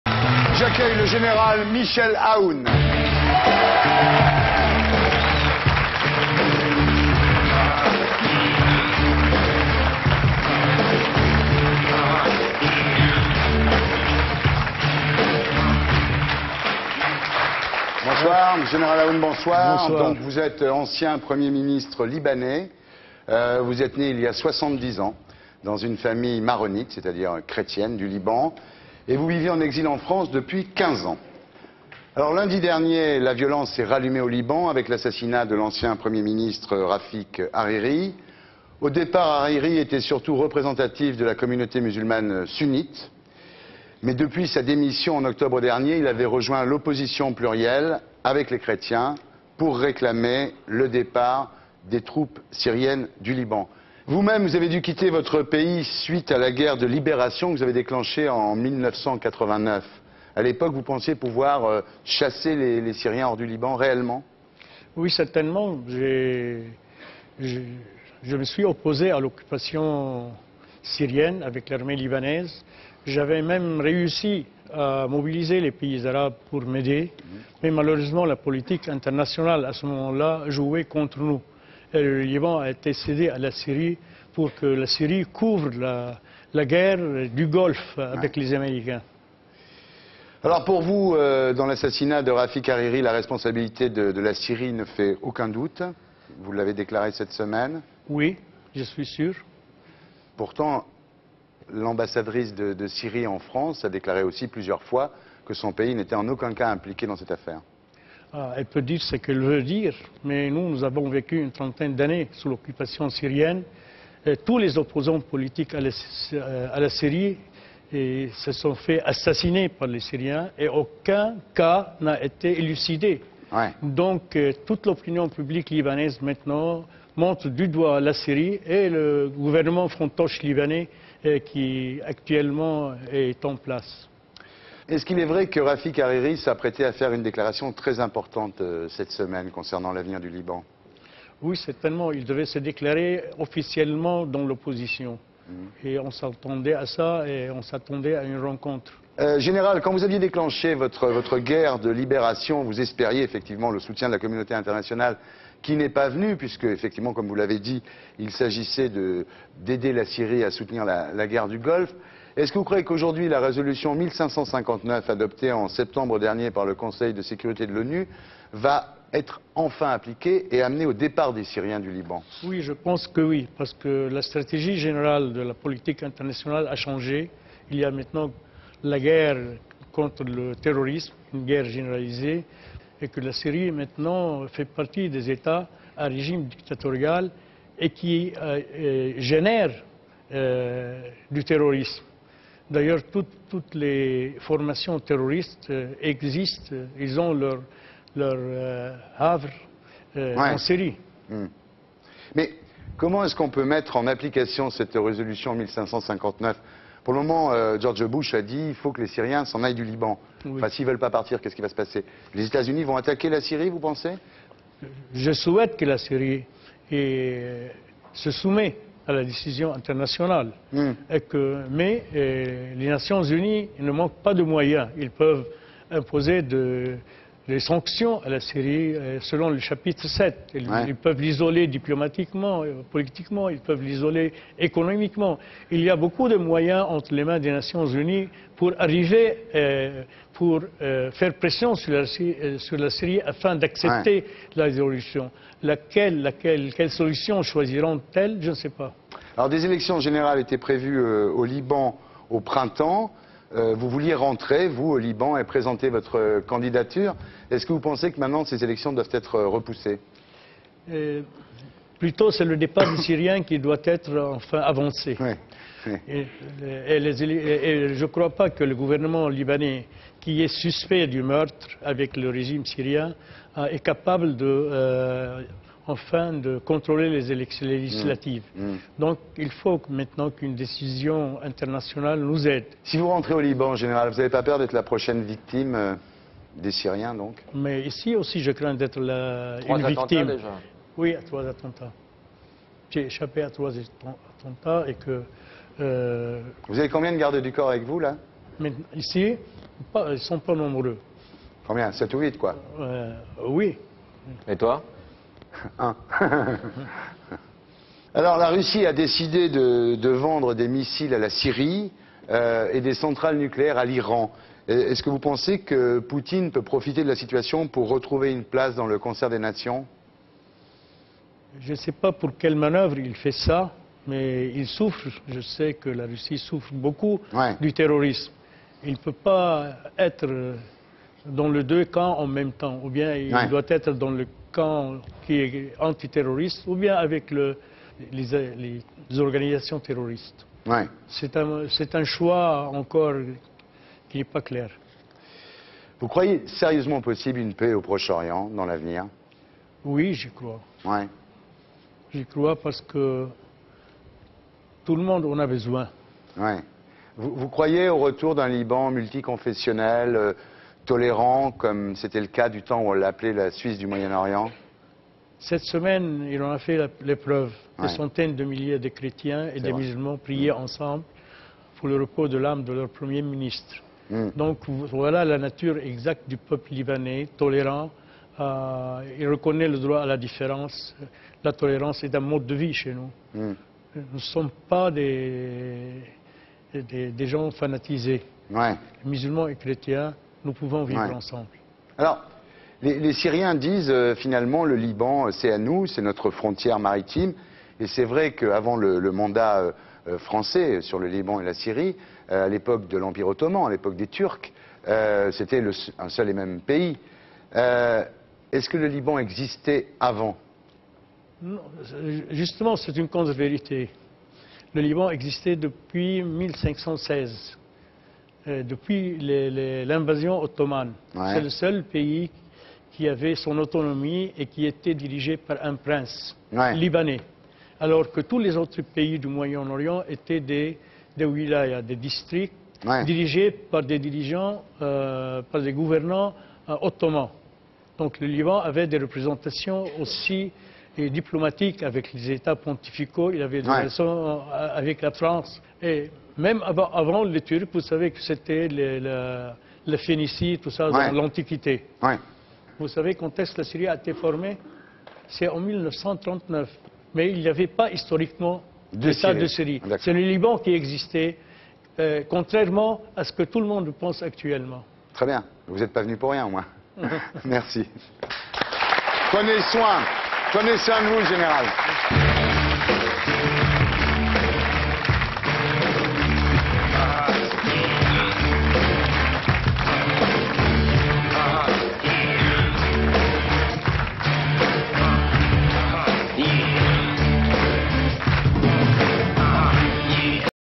Interview général Michel Aoun - Archive INA
Thierry ARDISSON reçoit l'ancien premier ministre libanais, le général Michel AOUN. Ils évoquent son exil en France depuis quinze ans et les tensions actuelles au Liban avec l'assassinat de l'ancien premier ministre Rafic Hariri. Images d'archive INA